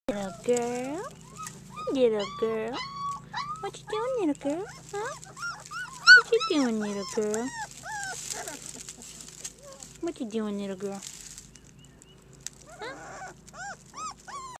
Princess Leias Miss Pink Sandy Beaches
Princess Leias Miss Pink Sandy Beaches is an absolutely gorgeous silver sable female puppy that is very affectionate and loving.